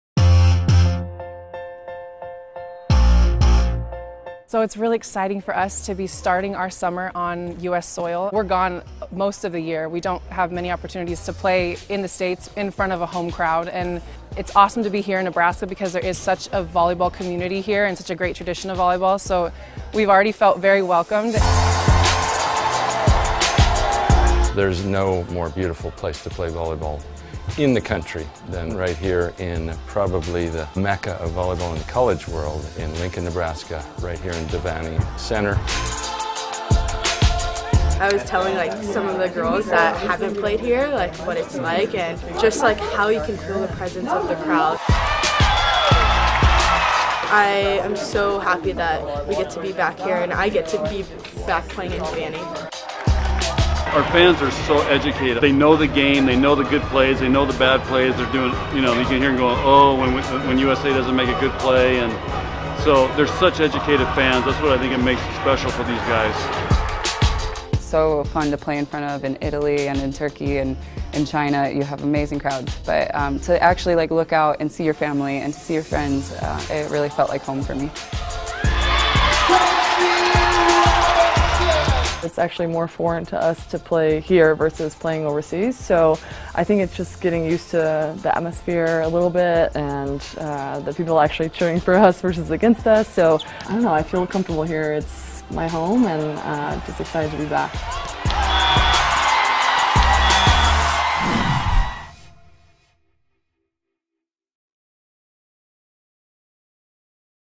Homecoming: Team USA Felt Right At Home In Lincoln
The United States opened VNL play on their home soil in the tradition-rich confines of the University of Nebraska.